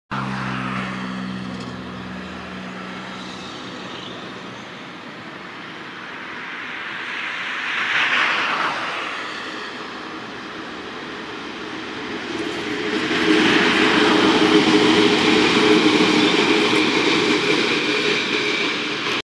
Light locos passing by the Sockburn overbridge
This is the light locos coming back from Rolleston after dropping off wagons there. It was filmed up on the Main South Road overbridge in Sockburn.